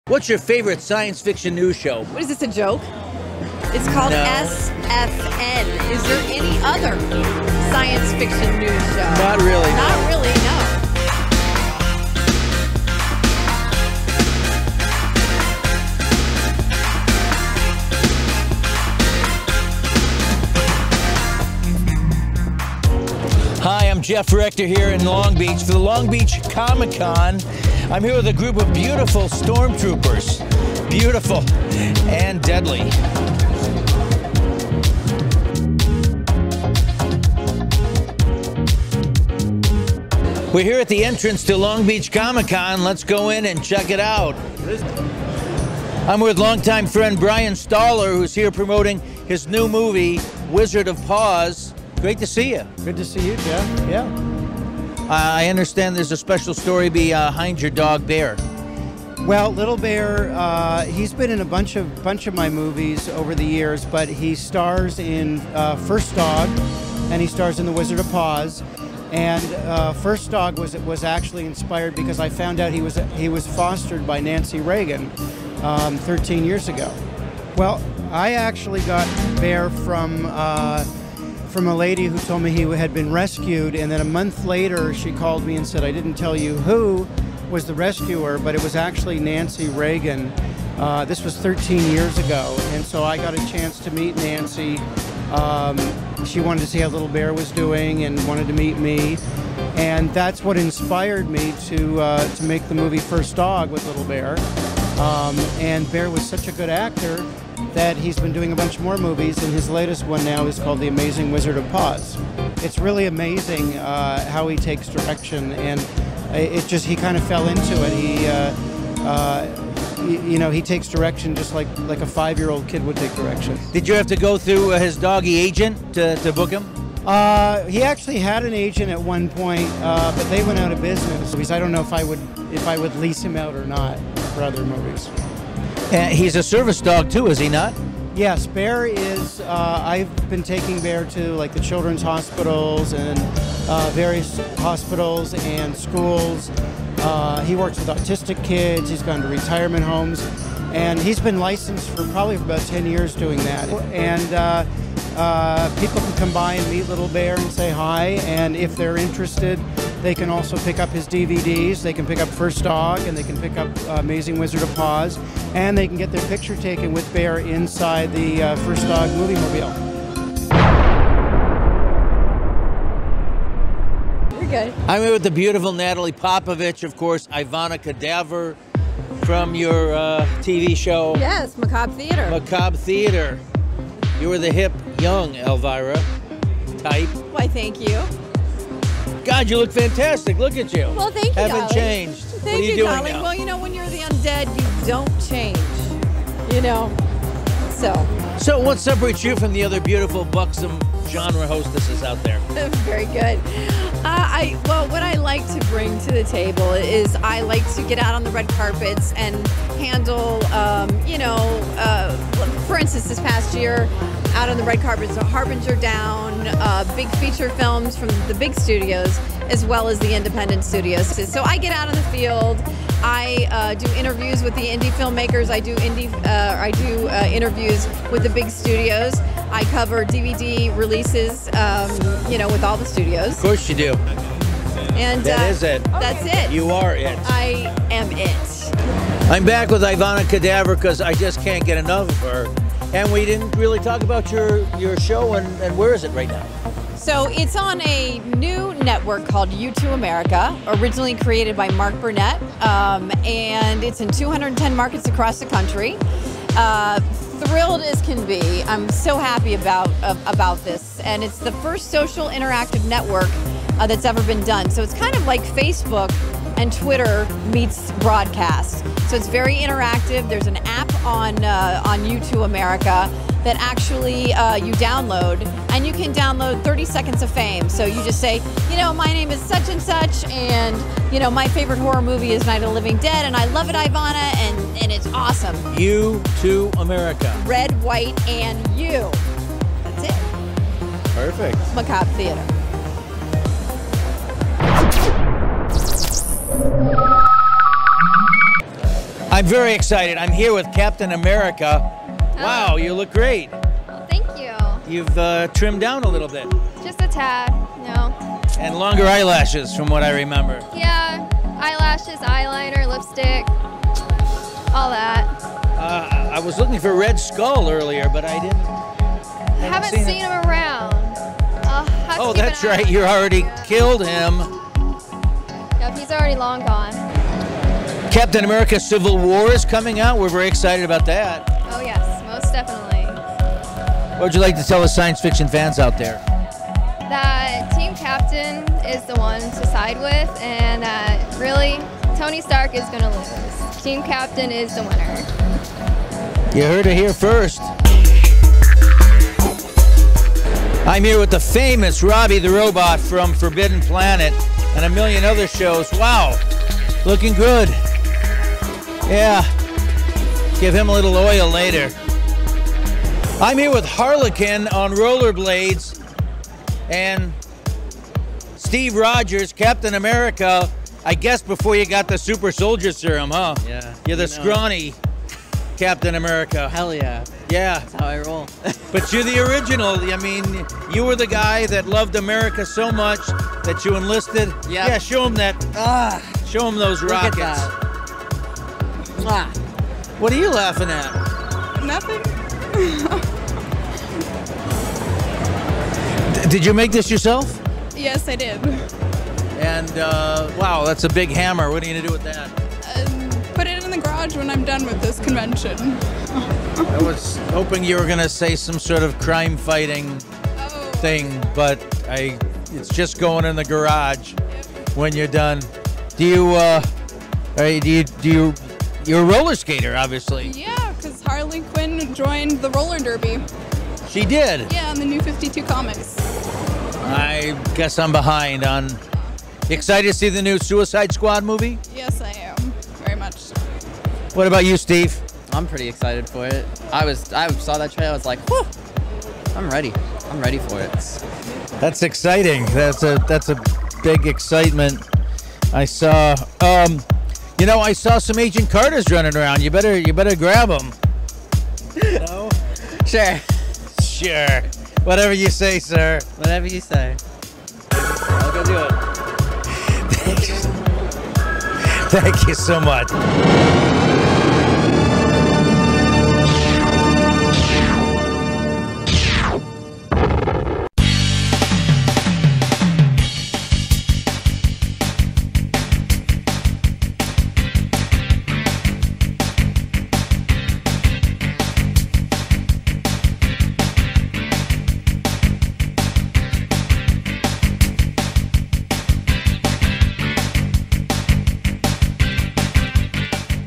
SFN-Science Fiction News (c): Coverage of 2016 Long Beach Comic Con
SFN visits with Bryan Michael Stoller and his dog Little Bear. Little Bear staring in The Amazing Wizard of Paws and First Dog.  Ivonna Cadaver host of Macabre Theatre talks about her show and upcoming news
Jeff covers exhibits and talks with fans and cos players.
Credits:
Don Black-Creator and Executive Producer
Jeff Rector- SFN Host and Co-Executive Producer
Director of Photography and edited by Don Black